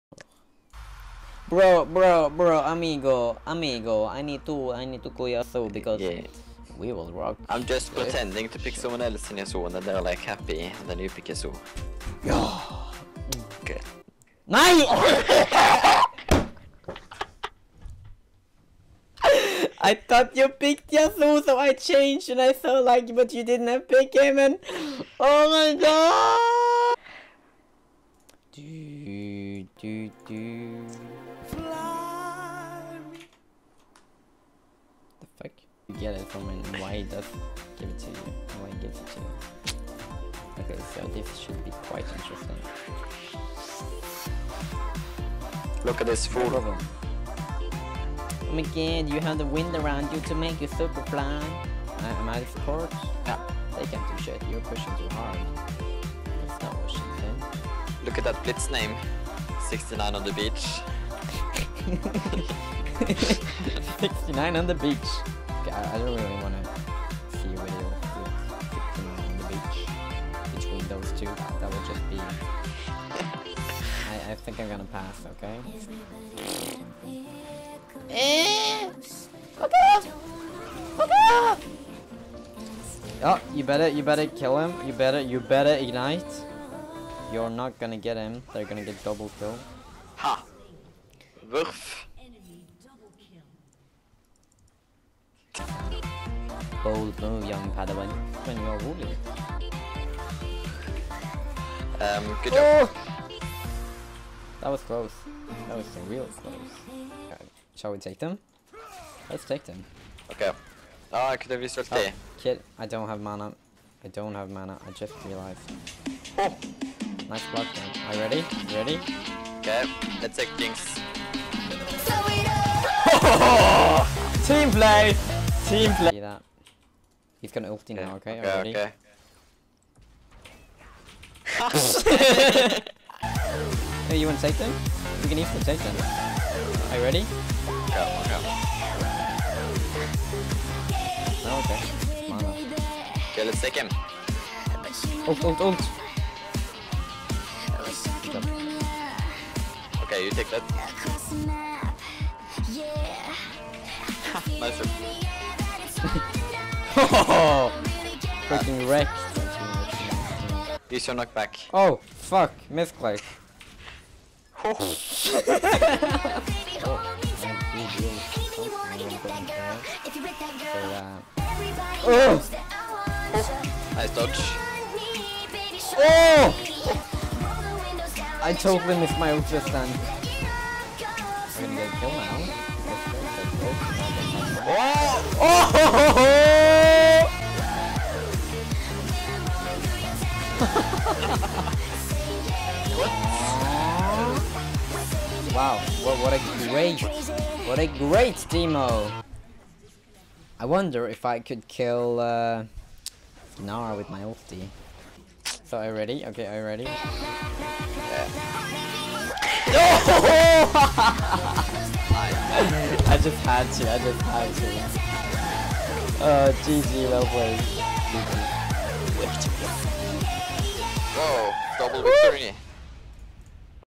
Zared - Best Moments #1 - Road To Silver V
Here's a funtage for u guys, i fckd up when picking champ, so i thought it was lost, which it was, or was it?